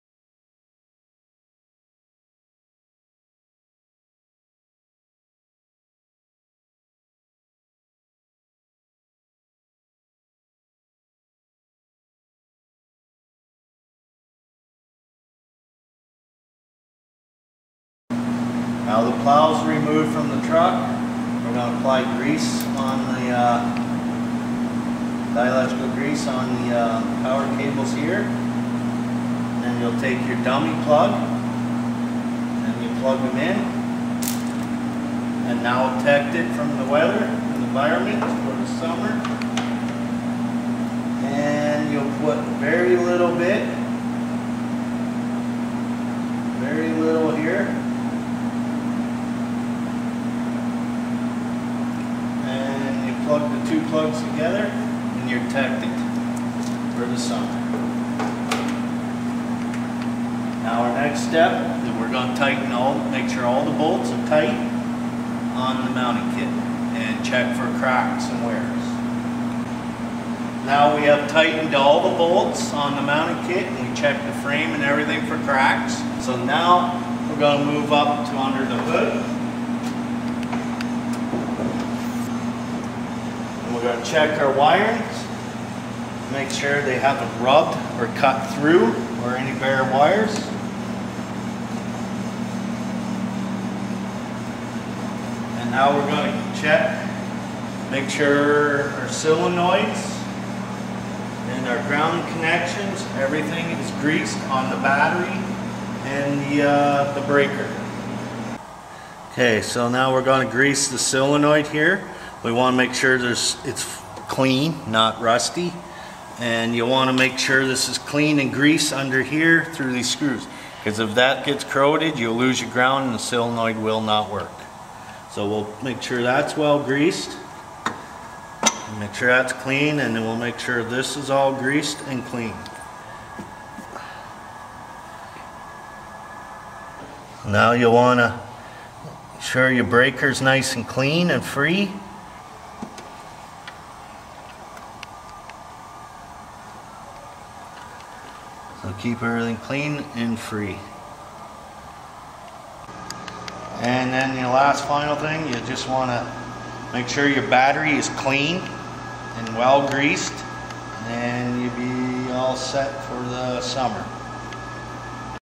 Arctic Snowplows - Preventive maintenance #4
Be sure to perform preventive maintenance on your plow at the end of the season in order that your plow is ready for the next winter. for more information.